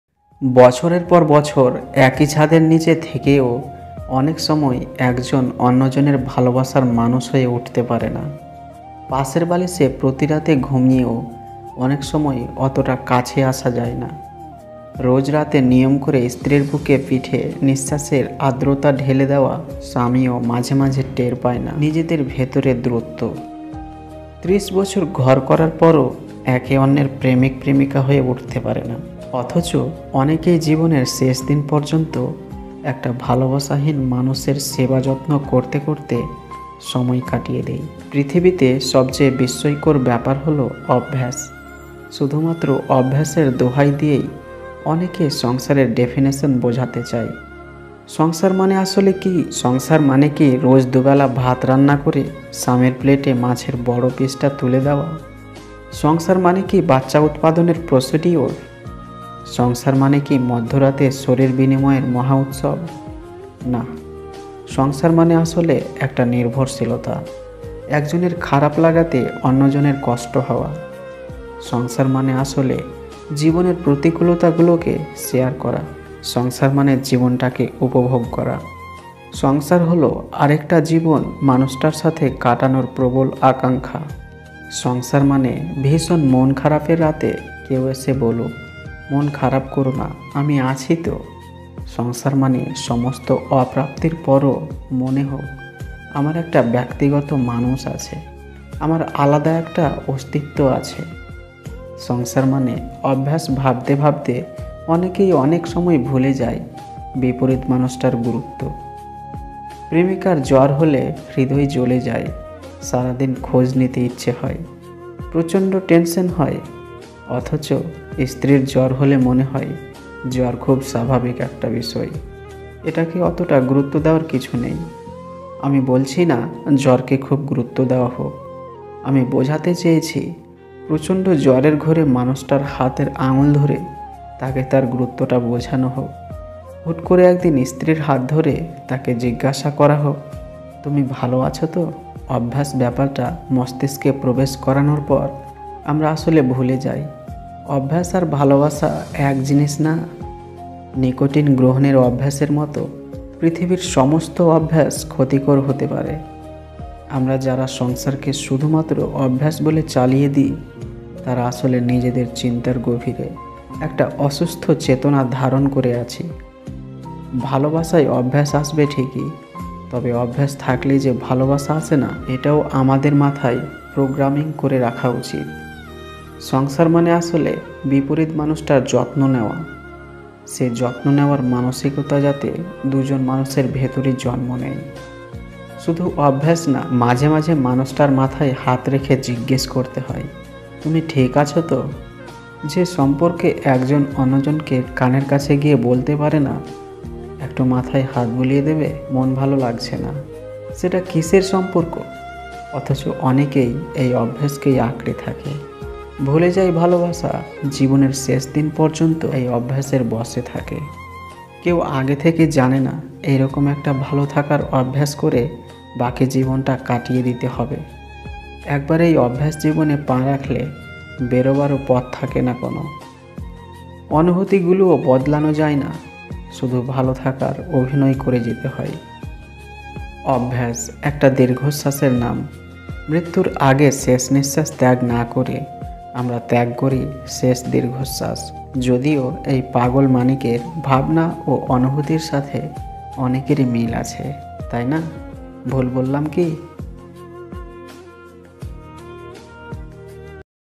Heart touching motivational quotes and story in Bangla
Heart touching short story and motivational quotes in bangla that will change your thought process & change your life surely.These quotes are of successful great men of the world like APJ Abdul Kalam, Shakespeare, Chanakya, Billgates and so on.